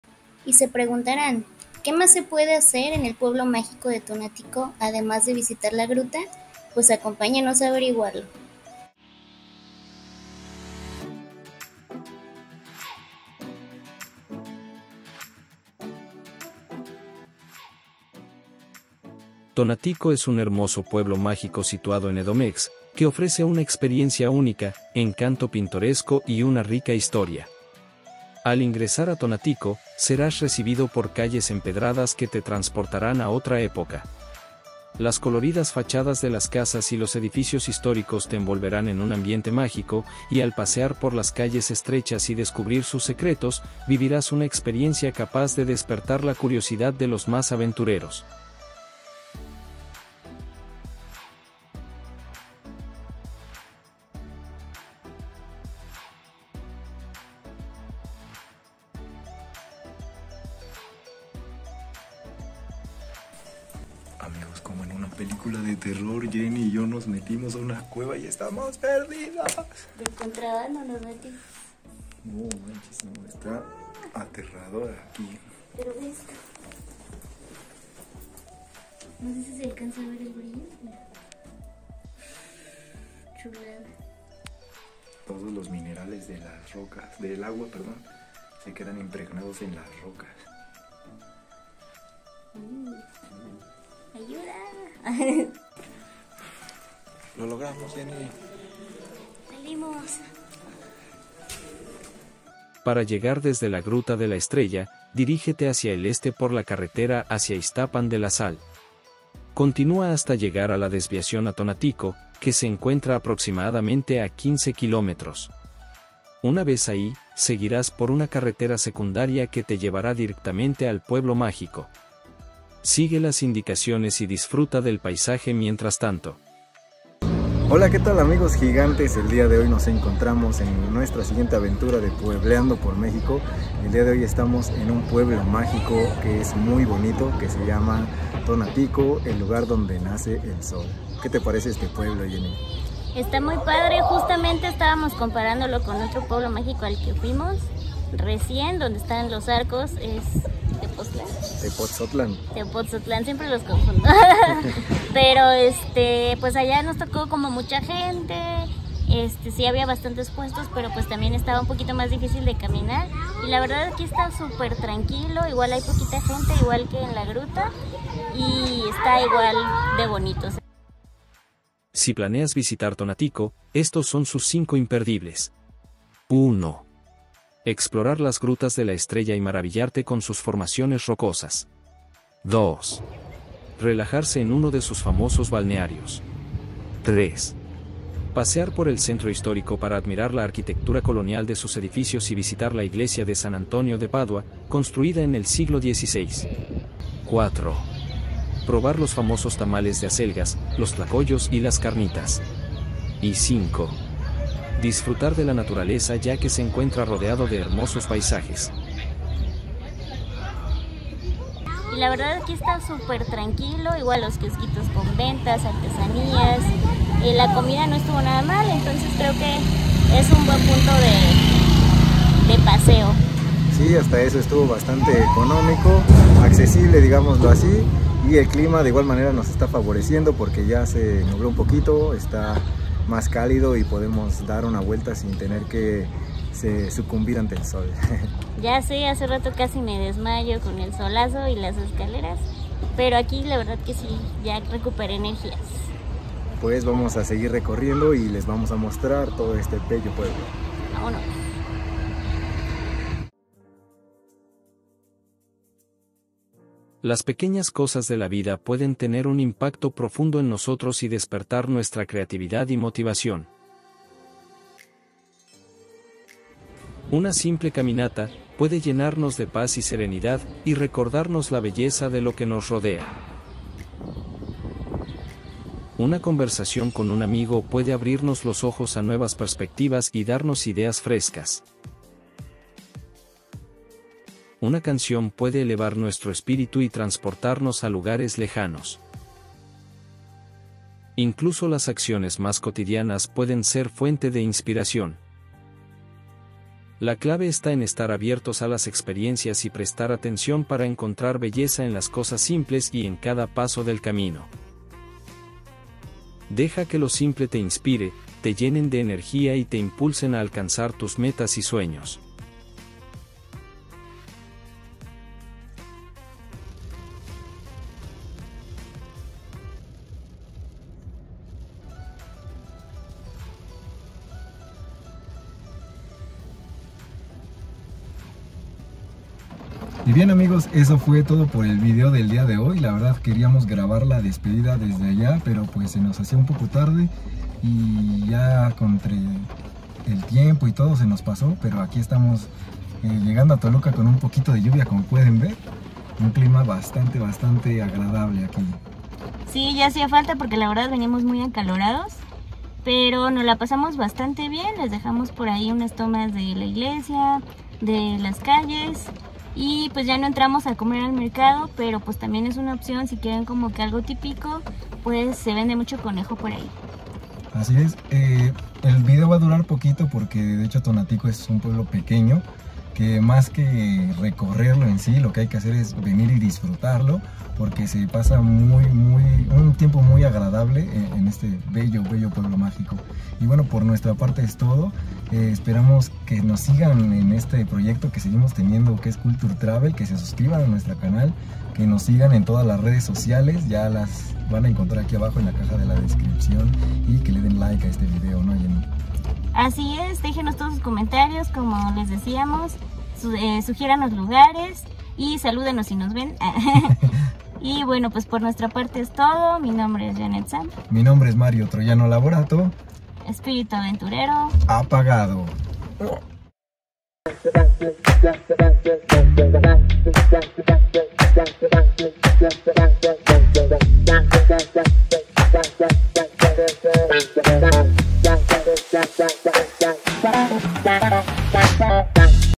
TONATICO Que hacer en este pueblo mágico
En esta ocasión acompáñanos a conocer un bello lugar que te encantara